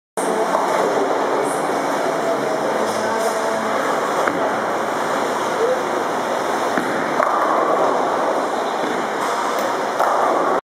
家族でボウリング 息子さん編 10/05/10
いつものように、ナムコ・ワンダーボウルで練習です。
サイドワインダー・14ポンドにもなれ、黒川プロ・中村プロ、そして川添プロからもアドバイスを受け、今日はアベレージ200ちょうどまできました＾＾（194・180・226・200）